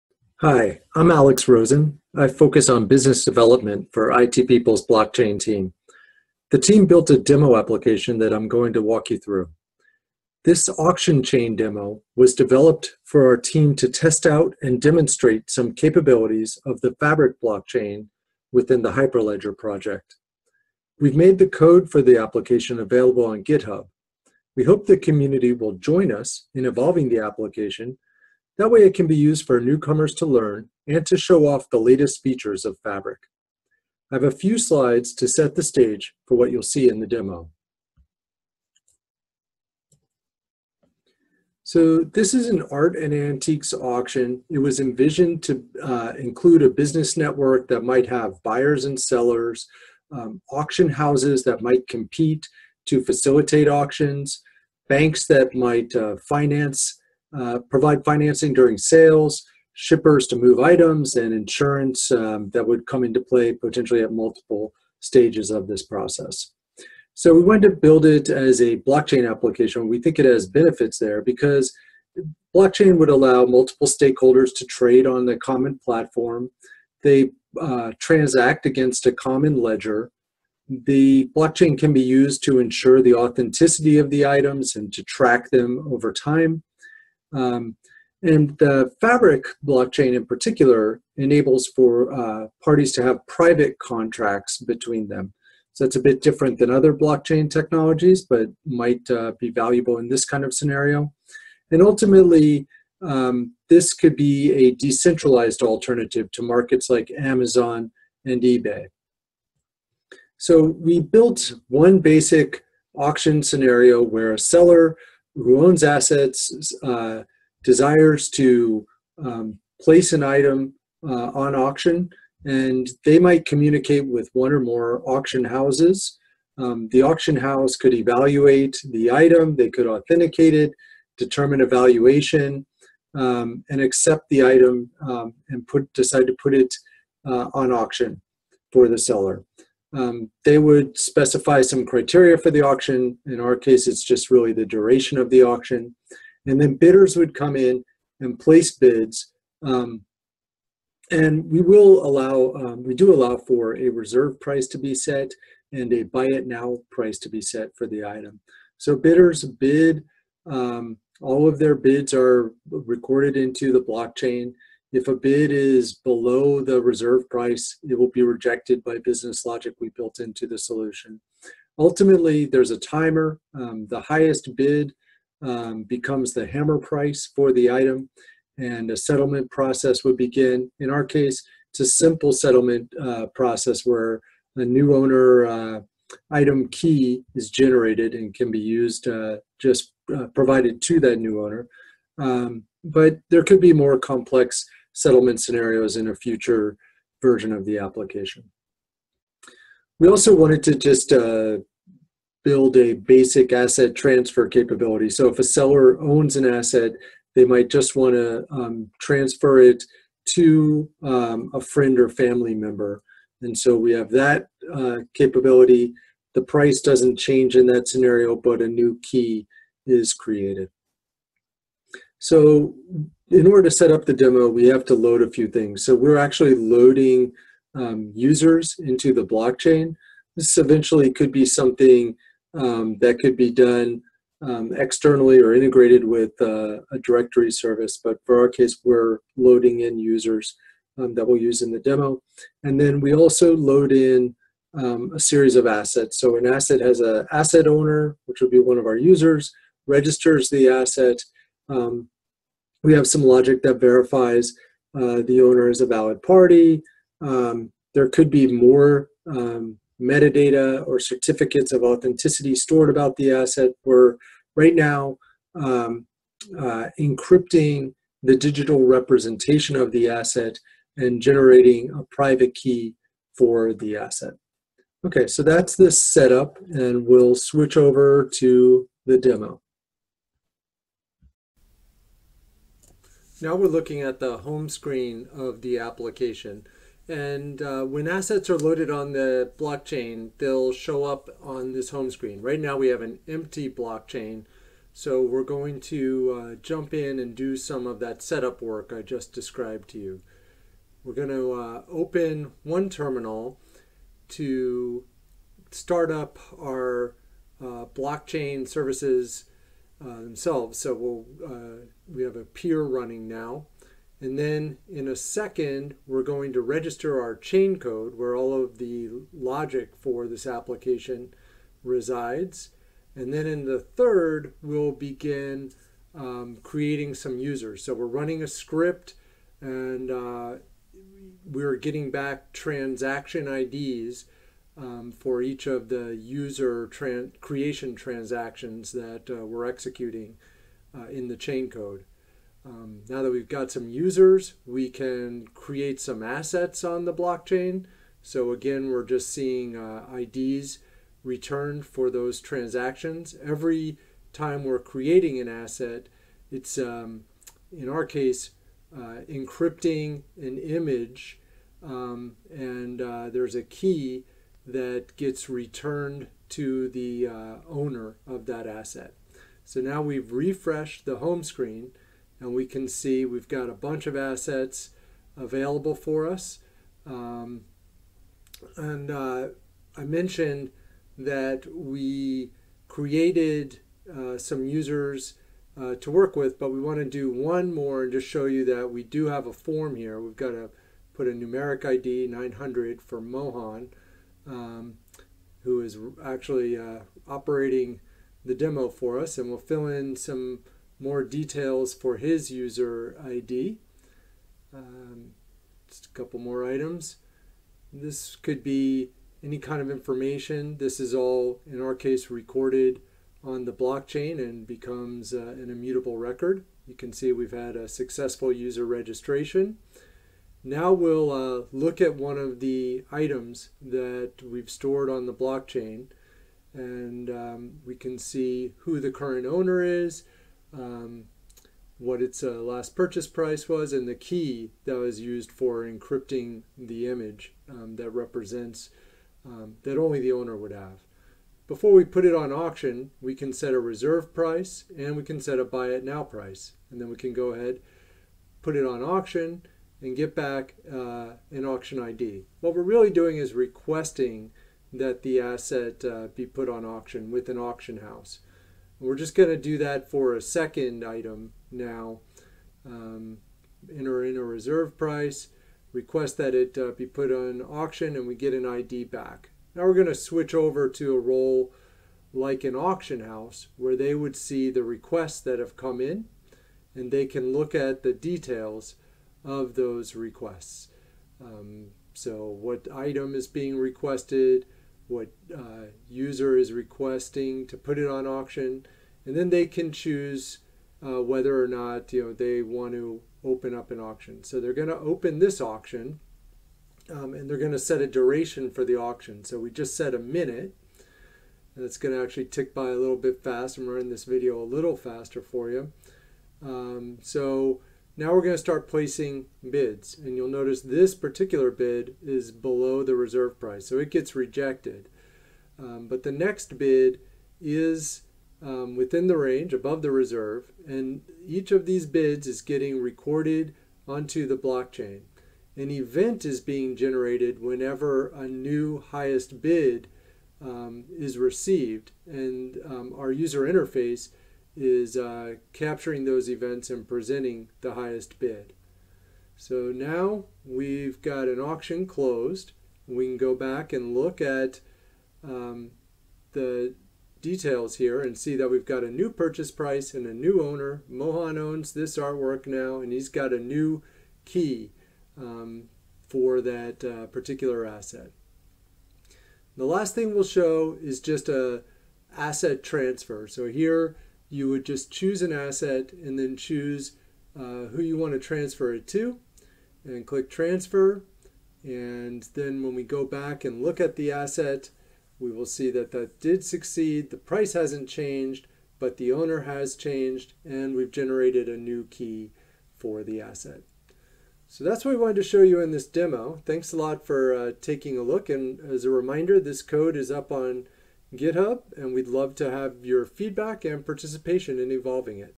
Hyperledger/Fabric Demo of an Auction Block Chain
Please see updated video on Youtube
This demo illustrates how one could develop chaincode and applications on the Hyperledger/Fabric, via an example that demonstrates auction of high value items , where multiple participants can work as a consortium taking advantage of the network effect. This demo is build on Hyperledger/Fabric v0.4. It is compatible with v0.5. We will soon have a Hyperledger/Fabric v1.0 version of this demo application.